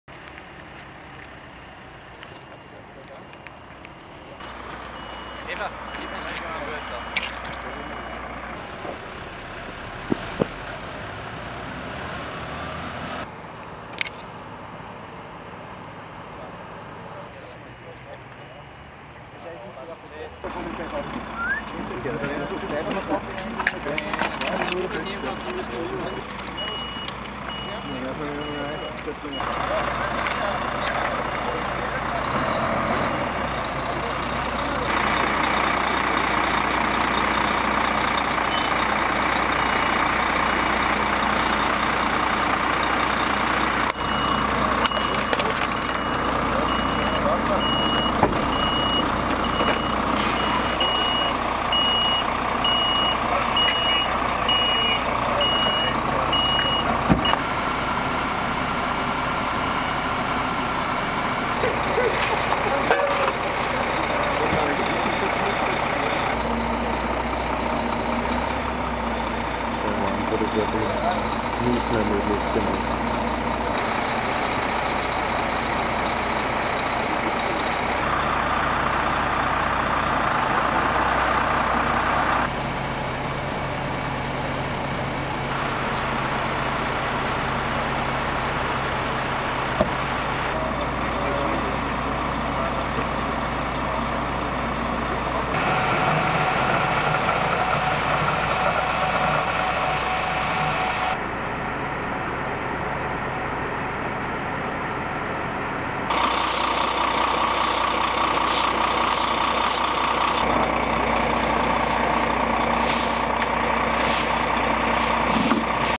Schwer- und Leichtverletzter bei Frontalkollision in Traun
Ein schwerer Verkehrsunfall Donnerstagnachmittag in Traun (Bezirk Linz-Land) forderte einen Schwer- und einen Leichtverletzten...